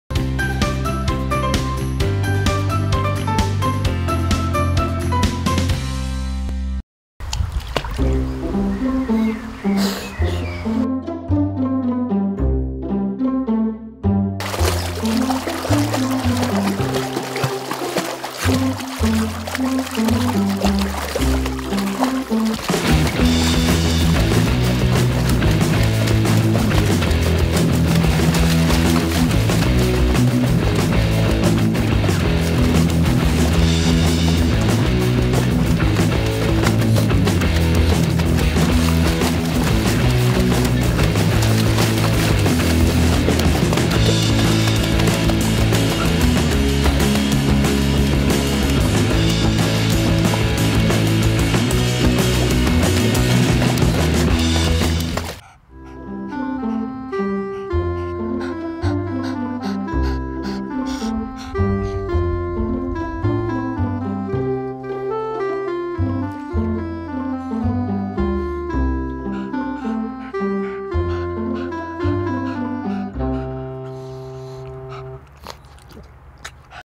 Mops lernt schwimmen | Pug gets used to swim in pool
An sich bin ich ja ziemlich wasserscheu, doch mein Spieltrieb führt mich immer wieder an das Schwimmbecken. Da ich schon ab und zu mal reingehüpft bin, muss ich natürlich auch schwimmen können. Frauchen übt das mit mir und gibt mir die nötige Sicherheit.